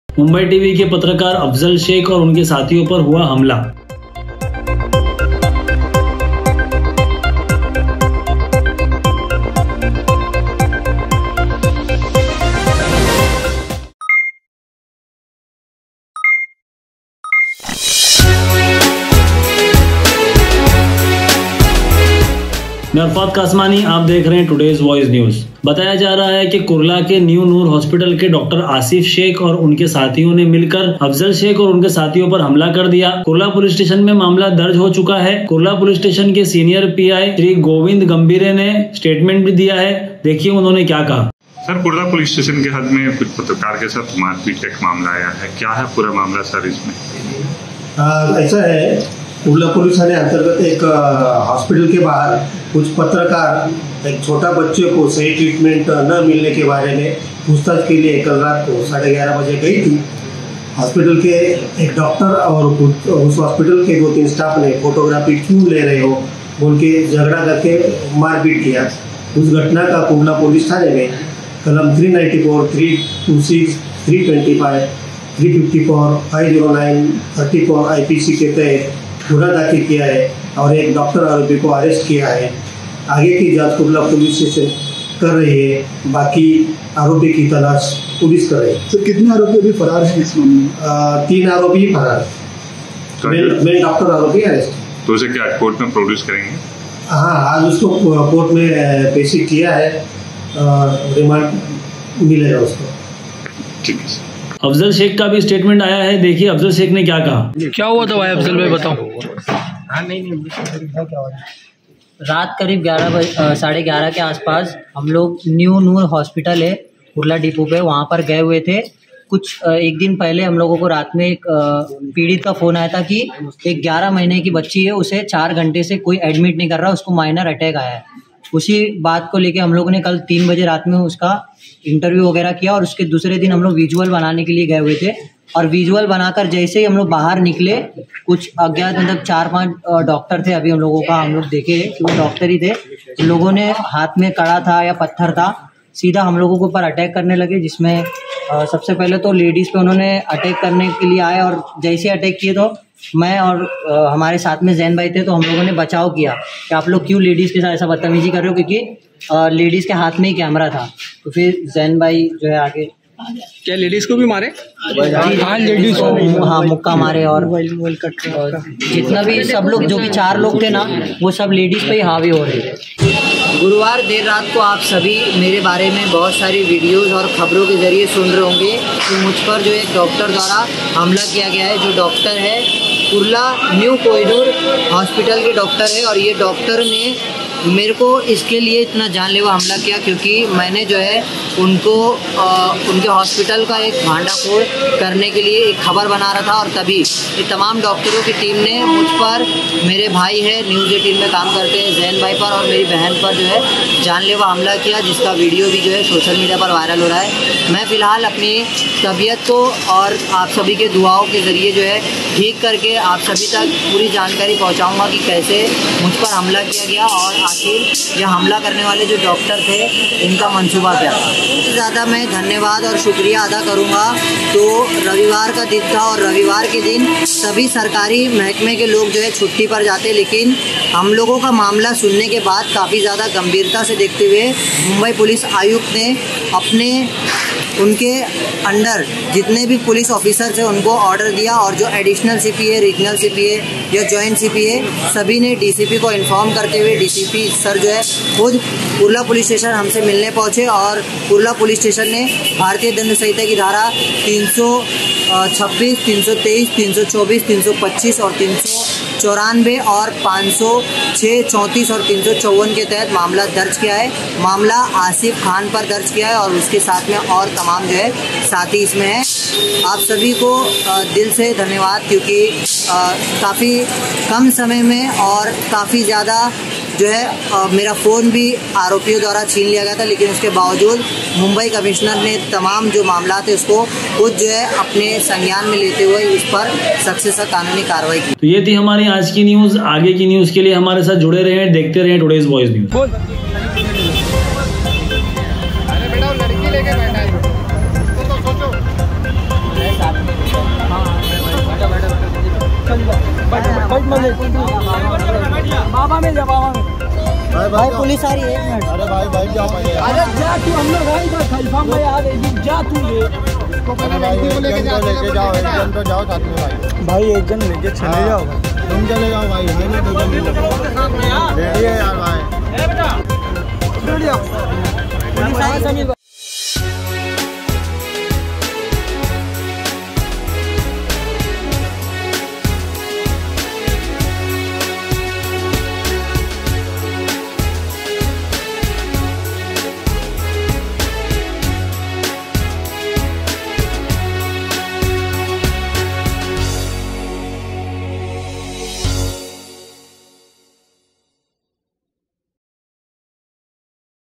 Mumbai TV Ke Patrakar Afzal shaikh Aur Unke sathiyo Par Hua Hamlaa | TODAYS VOICE NEWS
Mumbai TV Ke Patrakar Afzal shaikh Aur Unke sathiyo Par Hua Hamlaa.

Bataya Ja Raha Hai Ke Unpar Kurla Ki Ek New Noor Hospital Ke Dr Asif Aur Unke Sathiyon Ne Milkar Hamla Kiya Hai

Kurla Police Stn Mr FIR Darj Ho Gayi Hai

Poori Jankari Ke Liye Dekhiye Full Video